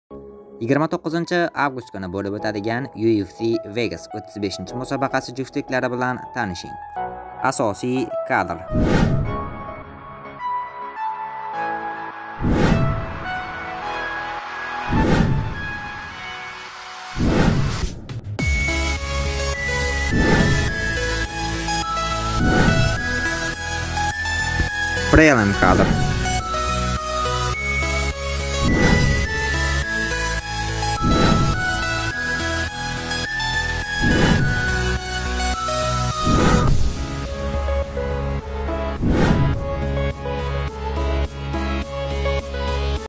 ТЕЗКОР!29-август куни бўлиб ўтадиган УФC Вегас 35 мусобақаси жуфтликлари билан танишинг
INSTAGRAM MANZILIMIZ OBUNA BO'LISHNI UNUTMANG